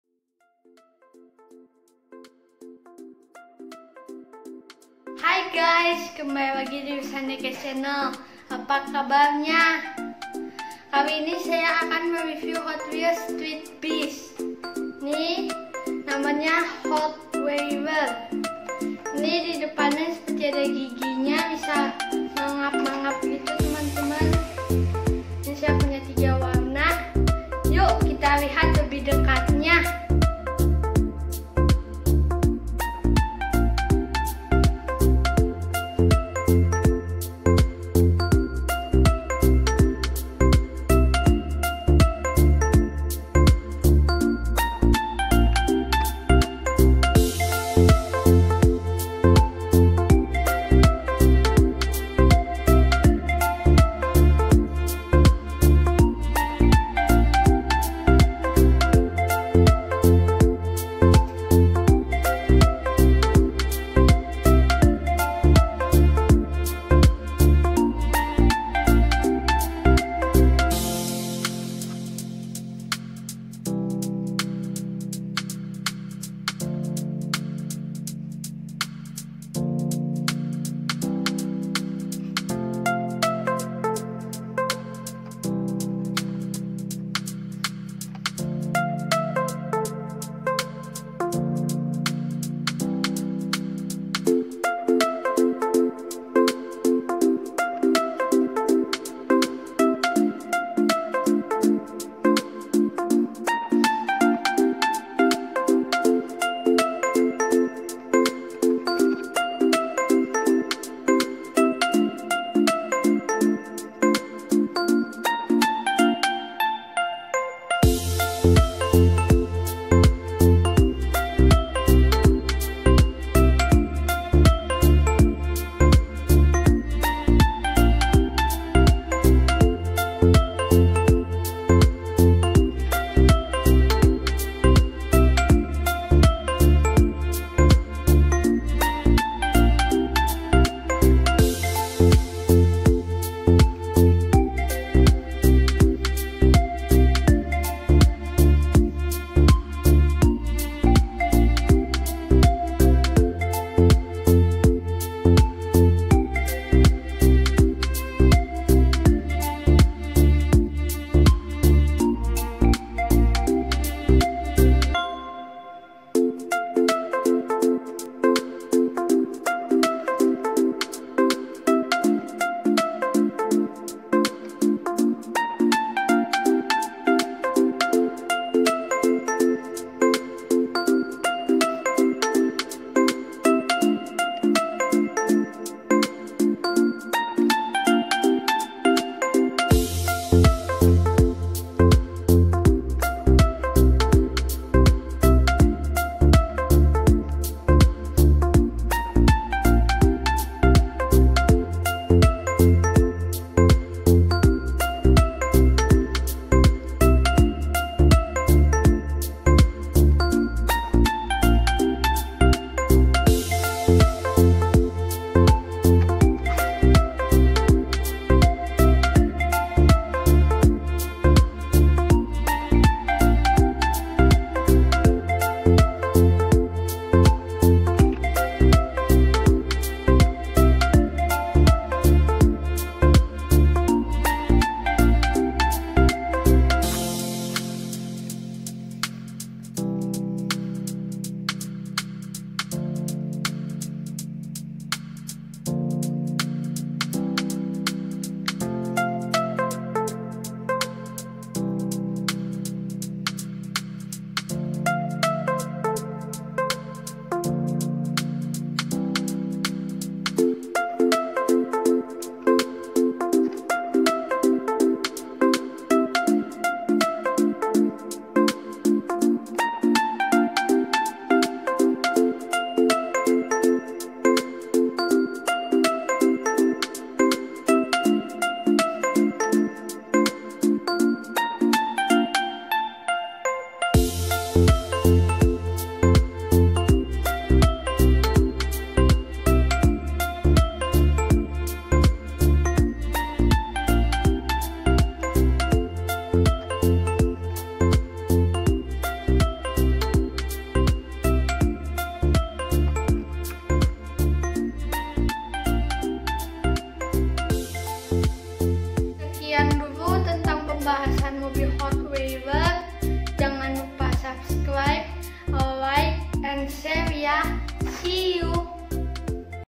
Hot Wheels Hotweiler street beasts collections review
Hotwheels Hotweiler
Debut Series : Street Beasts
Produced 2016
Designer : Julian Payne

Description
The Hotweiler is a Hot Wheels original model released in 2016. This model was obviously designed with a rottweiler dog in mind . The front of the models show some menacing teeth.  The dog is attached to a massive sturdy chain,which keeps the tires spinning. The name tag dangling at the back of the model says ''HW''. In the cargo bed a dog food bowl can be found. This model has a jaw-chomping motion when it is being rolled.

bisa dipesan di shopee.co.id/adolfoi
(jangan lupa klaim voucher free ongkir + cashbacknya)

Musician: @iksonmusic